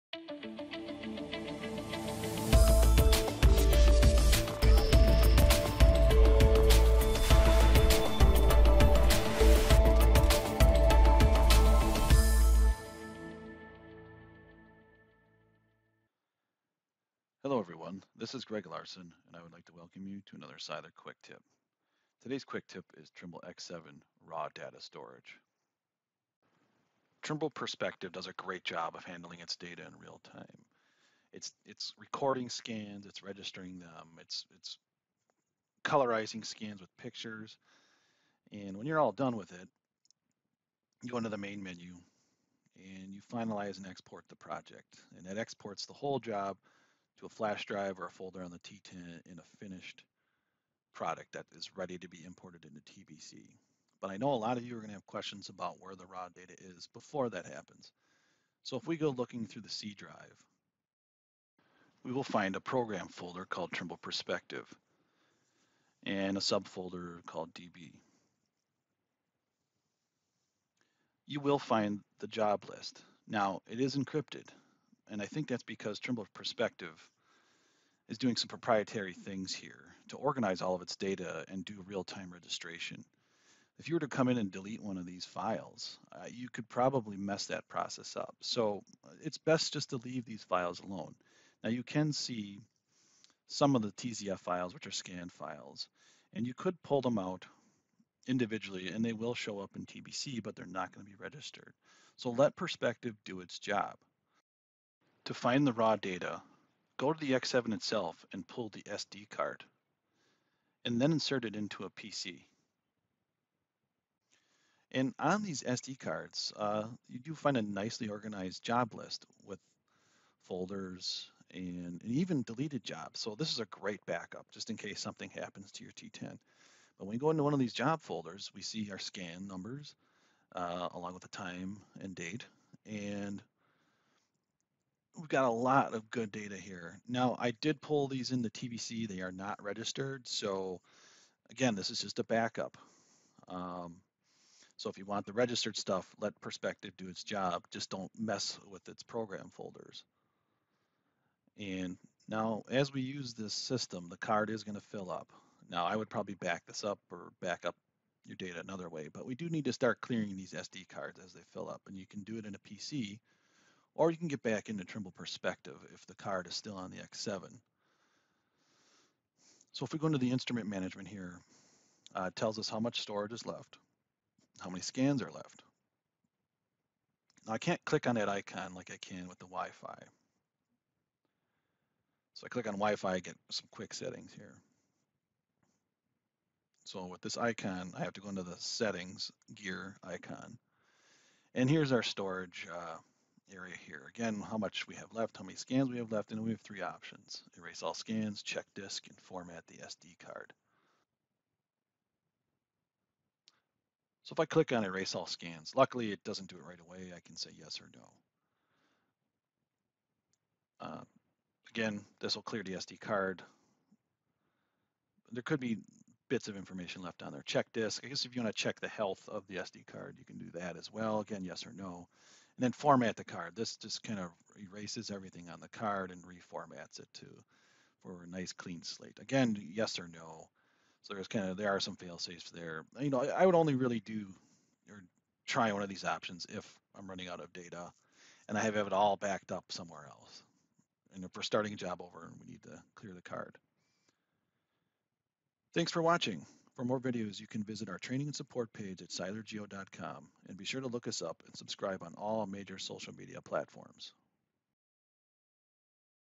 Trimble X7 Raw Data Storage and Trimble Perspective
Seiler quick tip on the new Trimble X7 Raw Data Storage and using Trimble Perspective and questions on where to find your raw data and more.